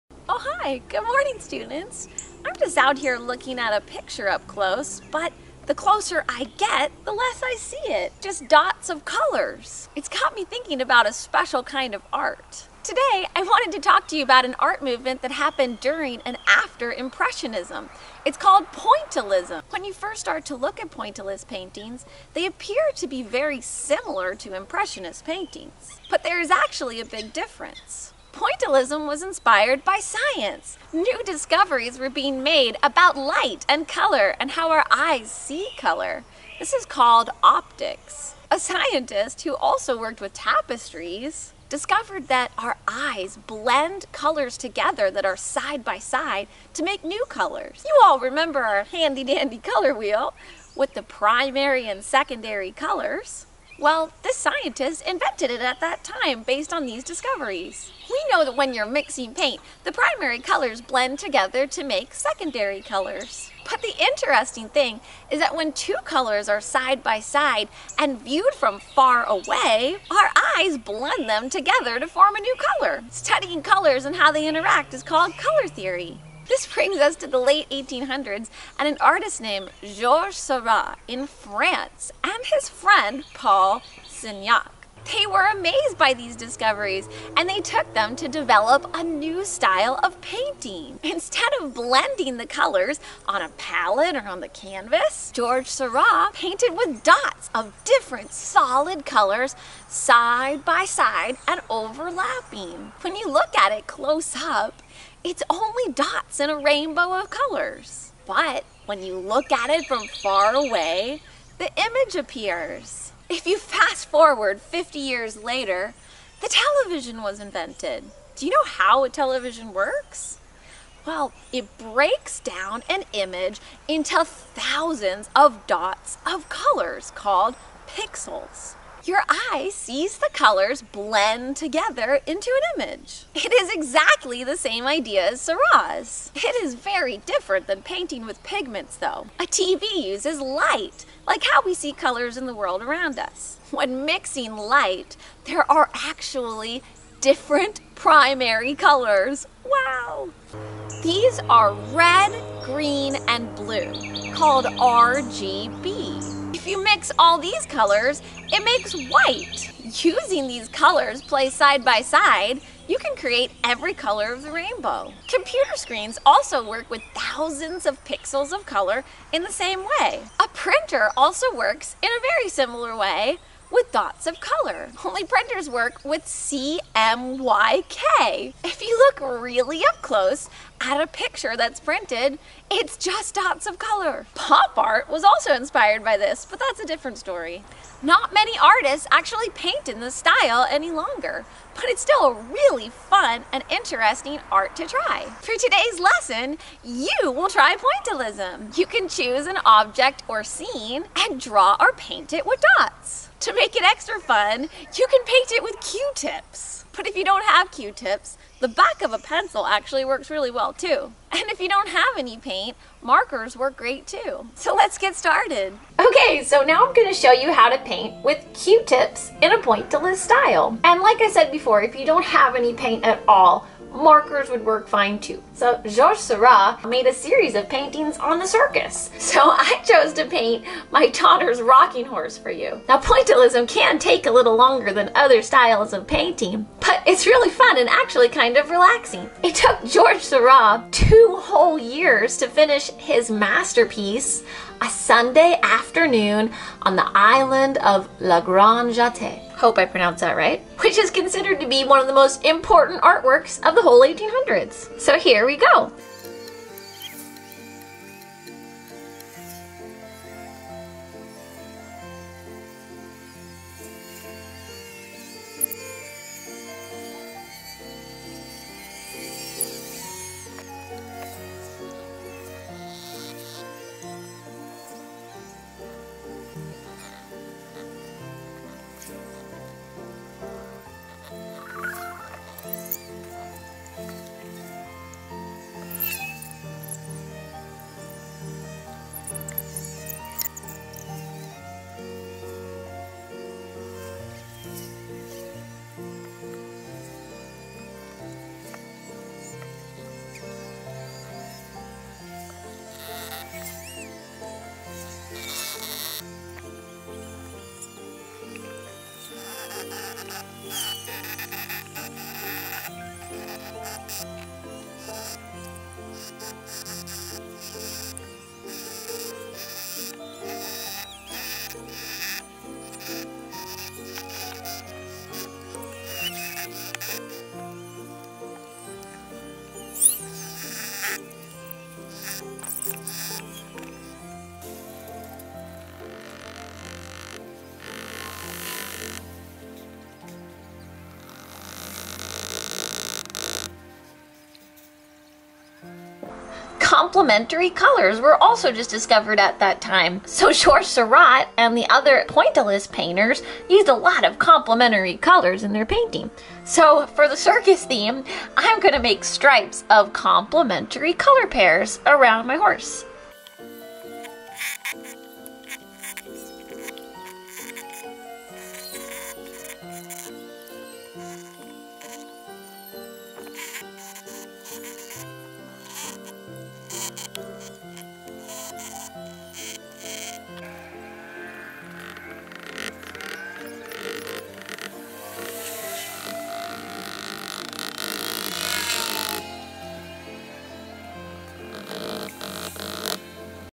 Pointillism to Pixels Lesson
Learn about pointillism and pixels, then make your own art in a pointillist style!